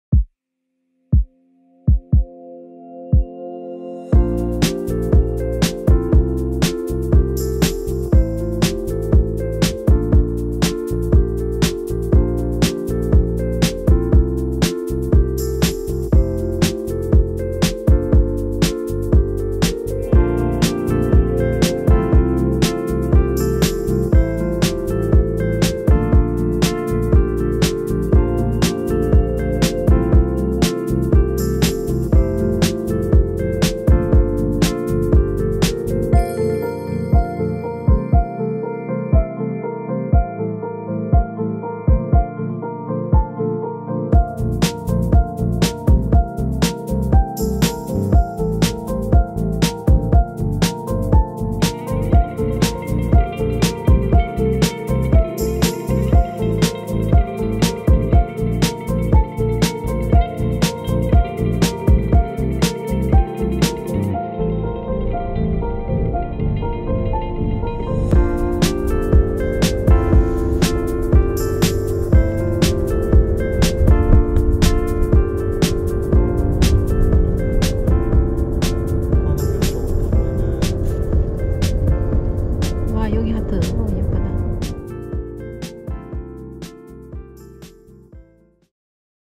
갤럭시 s10 야간 수동촬영
야간에는 자동보다 수동영상이 쓸만하네요